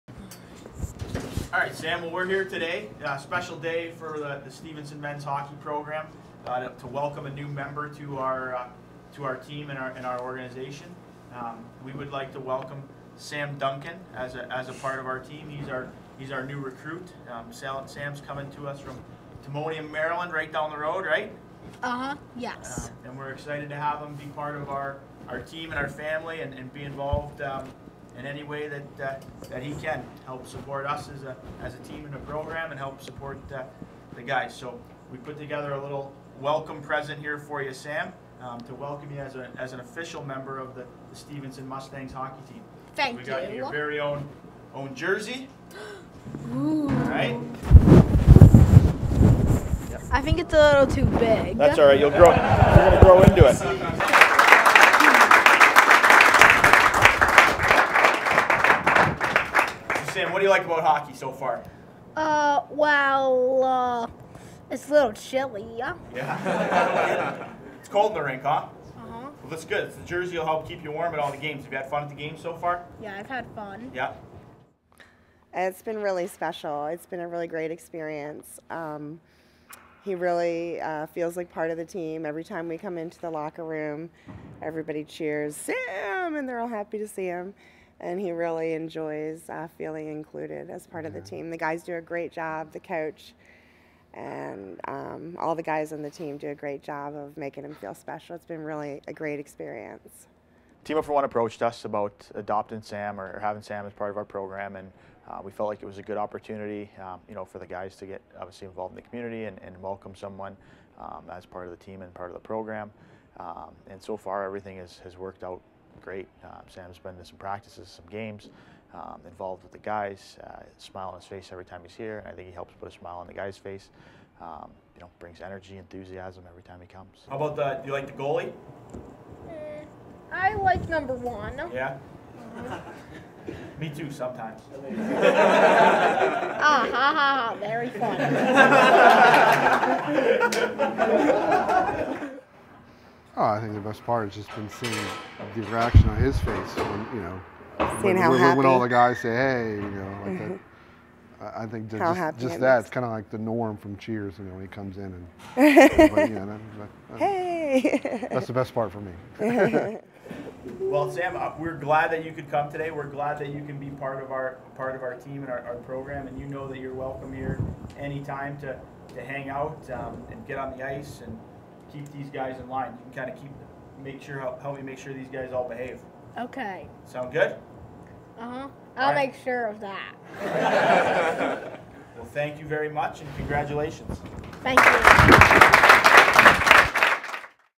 Men's Ice Hockey adds Sam Duncan
Stevenson Men's Ice Hockey added Sam Duncan to their roster in a partnership with the  Team Up for 1 Foundation.